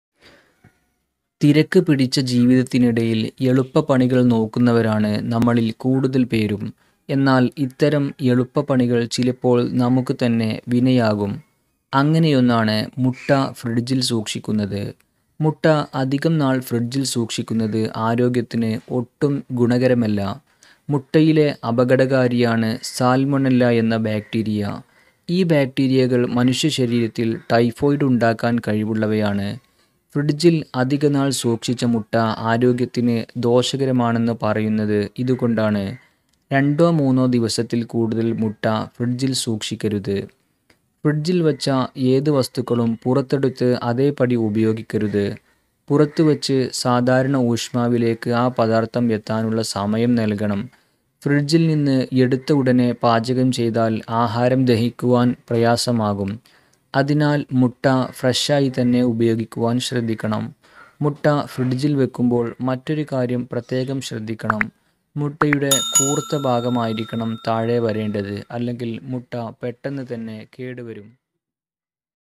മുട്ട ഫ്രിഡ്ജില്‍ വയ്ക്കുമ്പോള്‍ ഇക്കാര്യം പ്രത്യേകം ശ്രദ്ധിക്കണം | Health Tips Only Health Tips
തിരക്കുപിടിച്ച ജീവിതത്തിനിടയില്‍ എളുപ്പ പണികള്‍ നോക്കുന്നവരാണ് നമ്മള്‍ കൂടുതല്‍ പേരും. എന്നാല്‍, ഇത്തരം എളുപ്പ പണികള്‍ ചിലപ്പോള്‍ നമുക്ക് തന്നെ വിനയാകും. അങ്ങനെയൊന്നാണ് മുട്ട ഫ്രിഡ്ജില്‍ സൂക്ഷിക്കുന്നത്. മുട്ട അധികം നാള്‍ ഫ്രിഡ്ജില്‍ സൂക്ഷിക്കുന്നത് ആരോഗ്യത്തിനു ഒട്ടും ഗുണകരമല്ല.

മുട്ടയിലെ അപകടകാരിയാണ് സാല്‍മൊനല്ല എന്ന ബാക്ടീരിയ. ഈ ബാക്ടീരയകള്‍ മനുഷ്യശരീരത്തില്‍ ടൈഫോയിഡ് ഉണ്ടാക്കാന്‍ കഴിവുള്ളവയാണ്. ഫ്രിഡ്ജില്‍ അധികനാള്‍ സൂക്ഷിച്ച മുട്ട ആരോഗ്യത്തിനു ദോഷകരമാണെന്ന് പറയുന്നത് ഇതുകൊണ്ടാണ്. രണ്ടോ മൂന്നോ ദിവസത്തില്‍ കൂടുതല്‍ മുട്ട ഫ്രിഡ്ജില്‍ സൂക്ഷിക്കരുത്.

ഫ്രിഡ്ജില്‍ വച്ച ഏതു വസ്തുക്കളും പുറത്തെടുത്ത് അതേപടി ഉപയോഗിക്കരുത്. പുറത്തുവച്ച് സാധാരണ ഊഷ്മാവിലേക്ക് ആ പദാര്‍ത്ഥം എത്താനുള്ള സമയം നല്‍കണം. ഫ്രിഡ്ജില്‍ നിന്ന് എടുത്ത ഉടന്‍ പാചകം ചെയ്താല്‍ ആഹാരം ദഹിക്കാന്‍ പ്രയാസമാകും. അതിനാല്‍ മുട്ട ഫ്രഷായി തന്നെ ഉപയോഗിക്കാന്‍ ശ്രദ്ധിക്കണം. മുട്ട ഫ്രിഡ്ജില്‍ വയ്ക്കുമ്പോള്‍ മറ്റൊരു കാര്യം പ്രത്യേകം ശ്രദ്ധിക്കണം. മുട്ടയുടെ കൂര്‍ത്ത ഭാഗം ആയിരിക്കണം താഴെ വരേണ്ടത്. അല്ലെങ്കില്‍ മുട്ട പെട്ടന്ന് കേടുവരും.